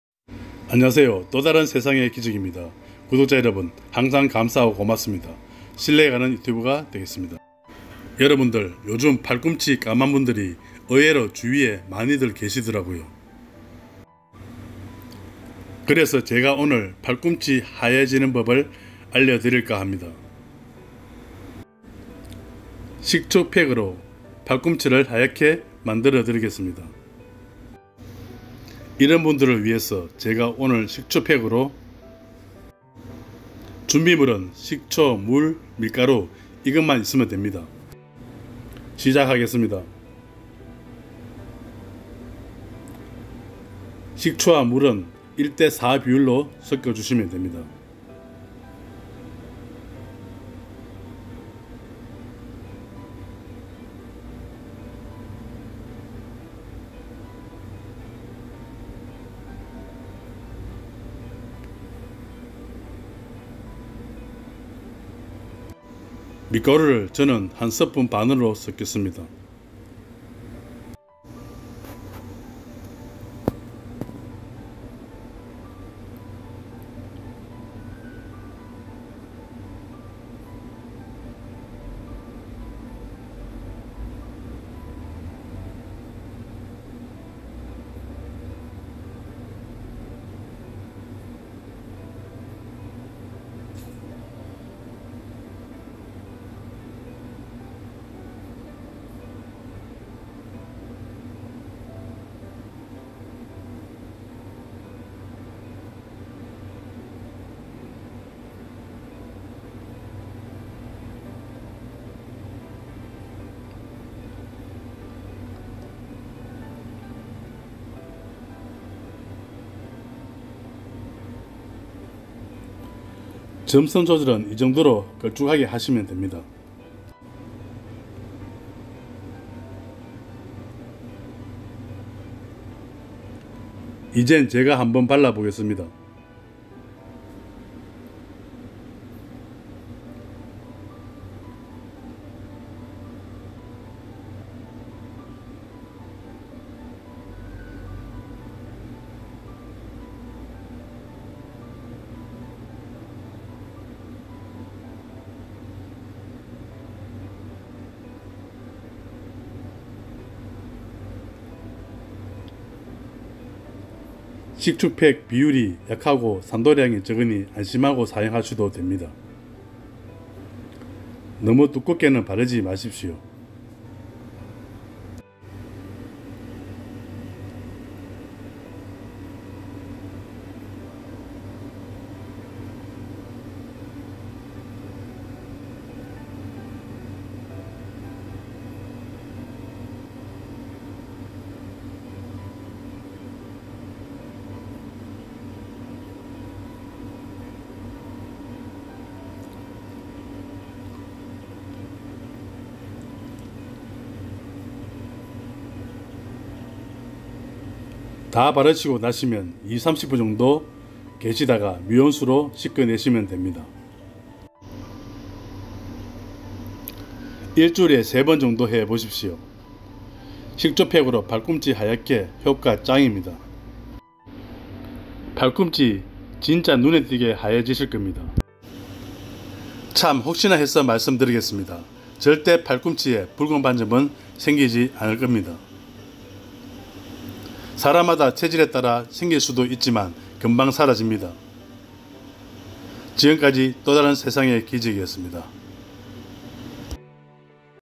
팔꿈치 하얘지는 법, 식초팩으로 효과 짱 입니다
팔꿈치 하얘지는 법
식초팩으로
식초 + 물   1:4비율로
하시면됩니다
점성조절은 밀가루로 하시면됩니다